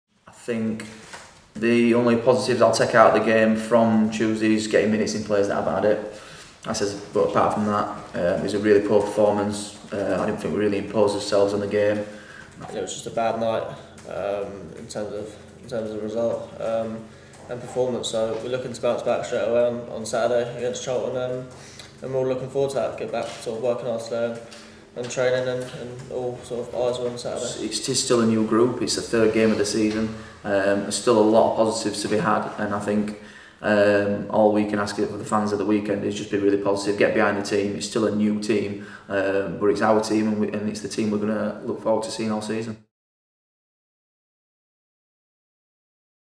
PRESS | Pre-Charlton Athletic
Head to iFollow Barnsley for the full pre-match thoughts of Dale Tonge and Conor Chaplin.